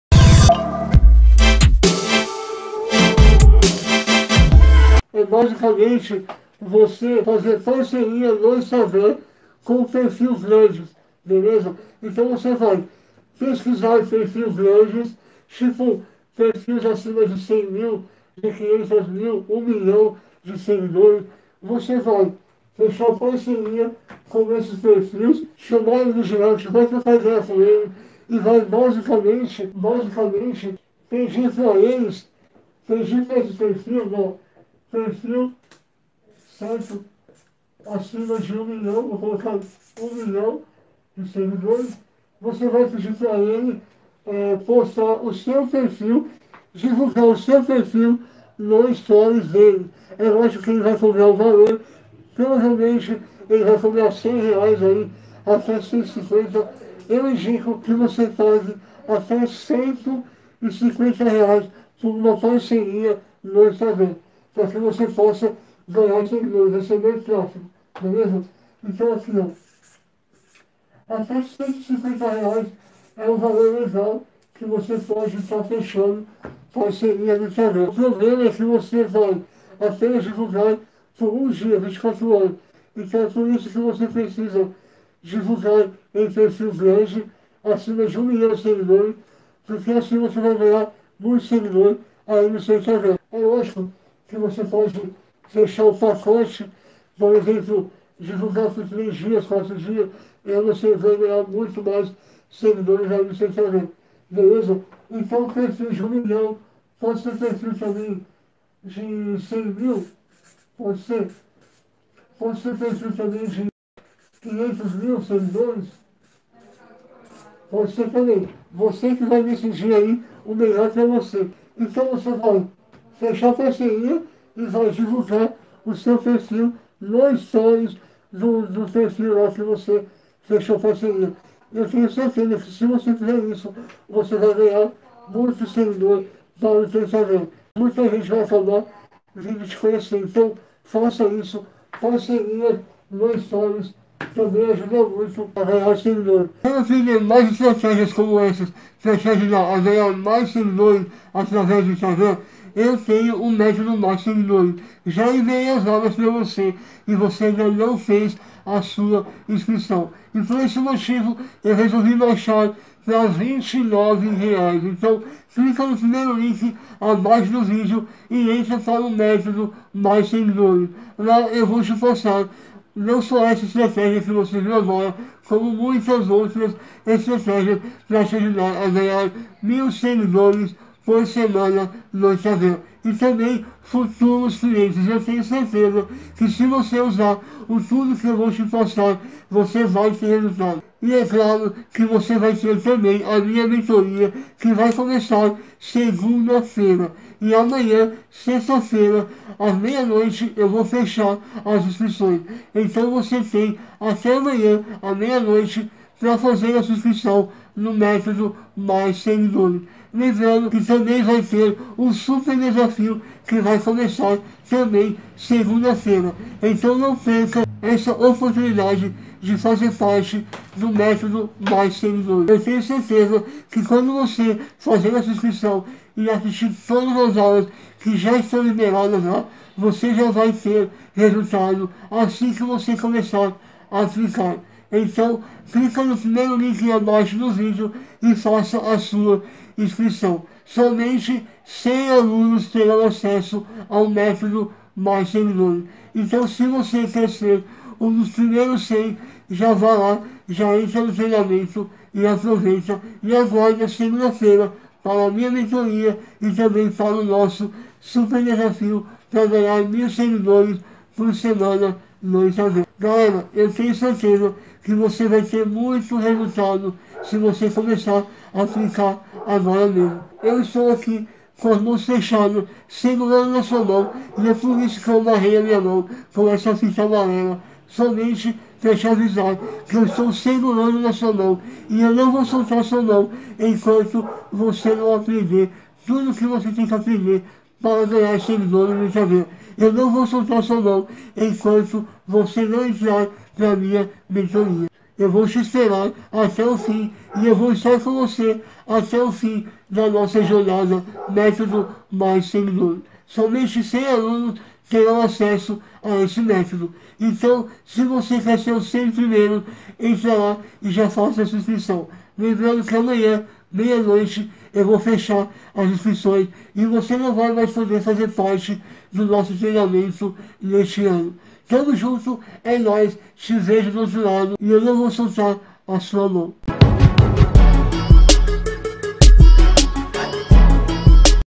FAZENDO PARCERIA NO INSTAGRAM PARA GANHAR SEGUIDORES E FUTUROS CLIENTES | FABIO LEMOS INSTAGRAM
❤️ ACESSA O MÉTODO + SEGUIDORES

(Links Importantes Para Acessar)

Nesse vídeo eu vou te ensinar uma super estratégia de ganhar seguidores no instagram usando a parceria no instagram